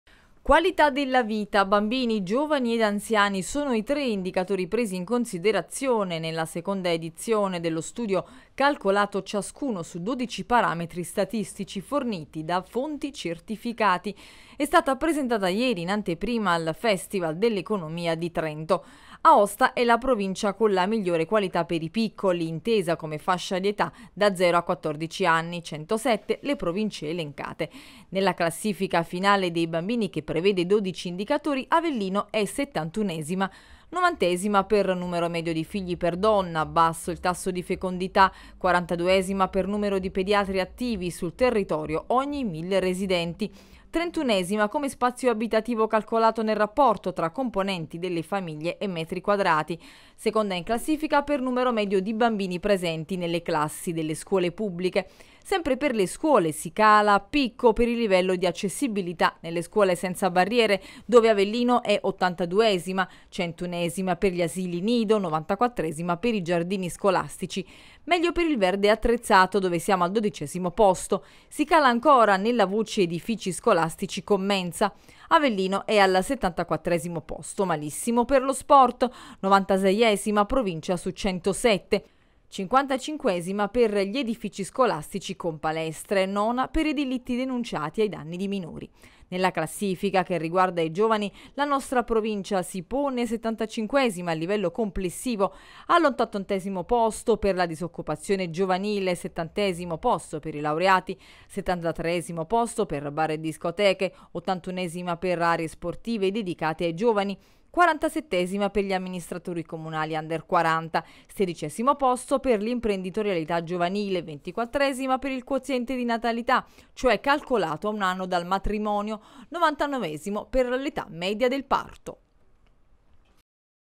Qualità della vita. Ad Avellino male i bambini, peggio ancora i giovani
Qualità della vita. Dallo studio del Sole 24 ore malissimo la nostra provincia per bambini e giovani. Giù in classifica per scuole accessibili, verde, aree sportive dedicate, bar e discoteche. 88esimi per la disoccupazione giovanile.